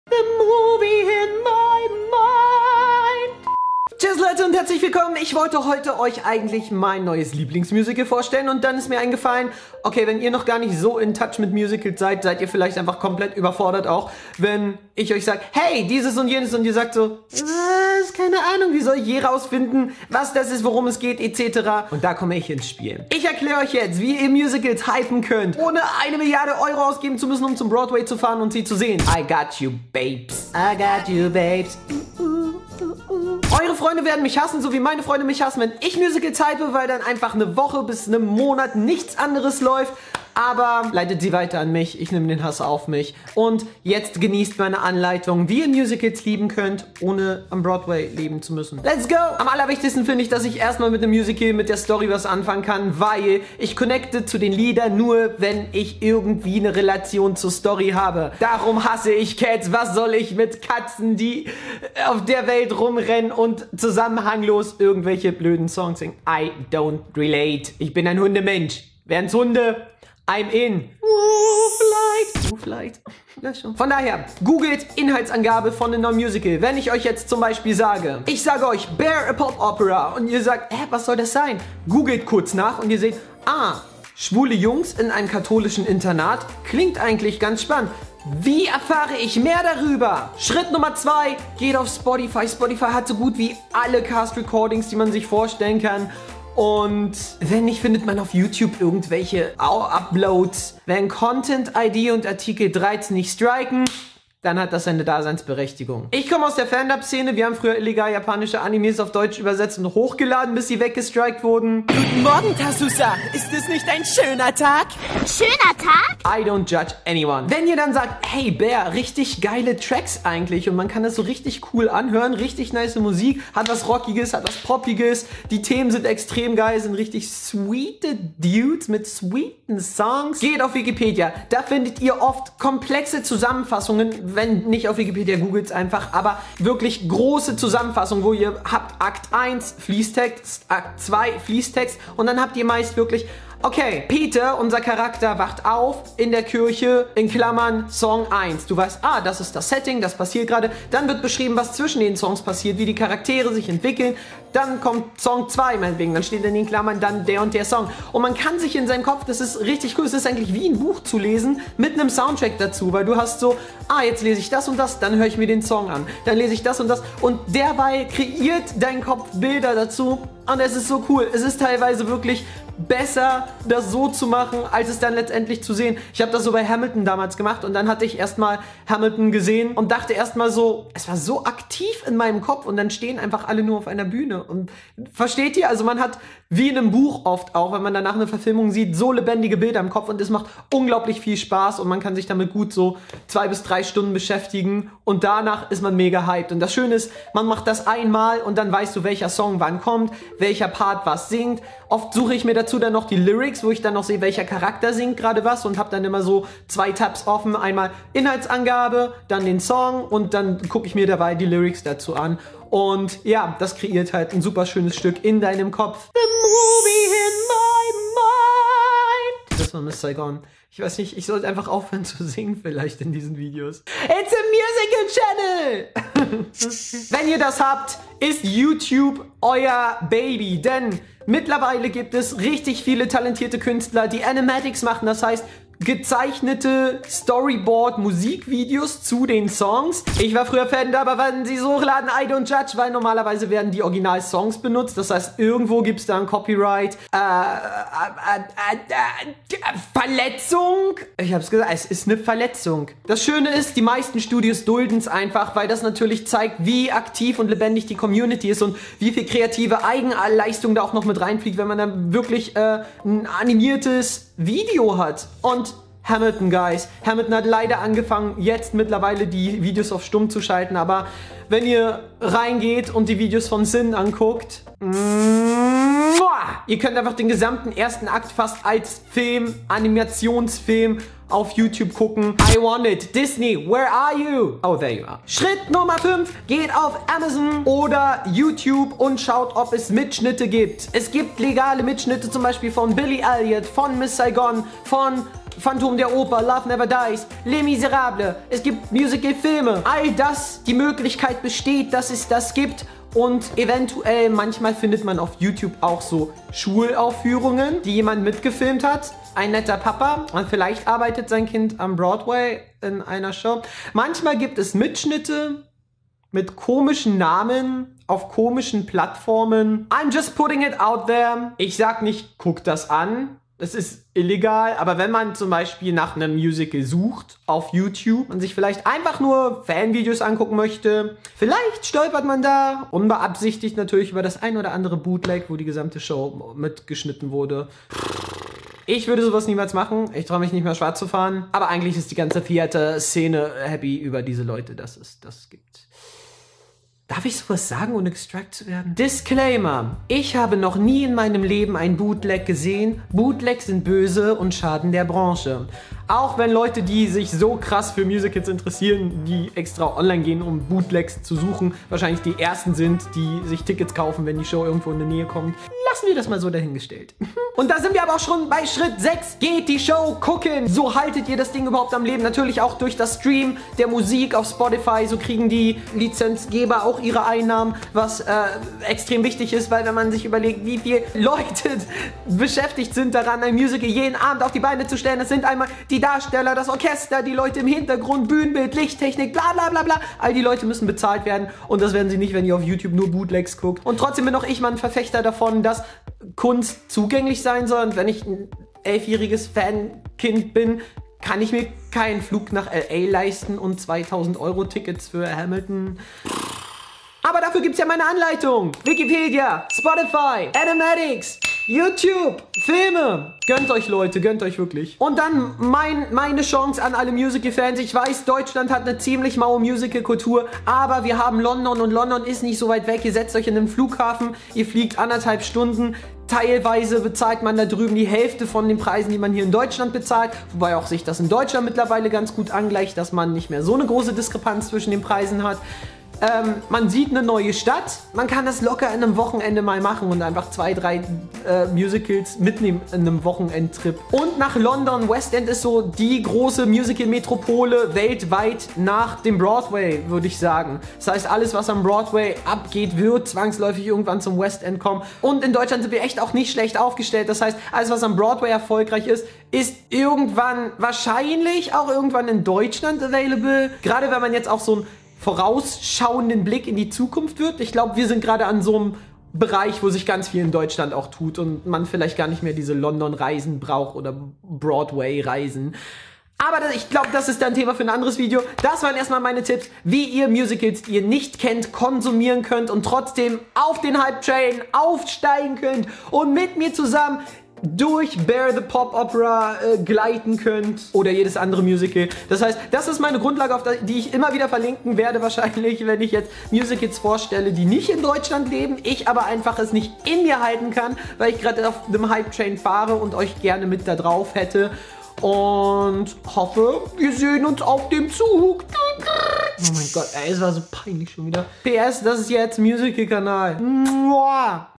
Jedes MUSICALS von ZUHAUSE aus 😱♥
In diesem Video erkläre ich euch, wie ihr euch von zu Hause aus jedes Musical gönnen könnt, um in den Hype zu kommen! :D

Yup, Musicals ist nicht mehr nur was für alte Leute und Familien, sondern es gibt mehr und mehr relevante Shows wie Heathers, Hamilton, Be More Chill, die auch zu Teens sprechen - Aber wie soll man da ran kommen, wenn man nicht am Broadway oder in London am West End lebt?
I'm gonna tell you, guuuys!!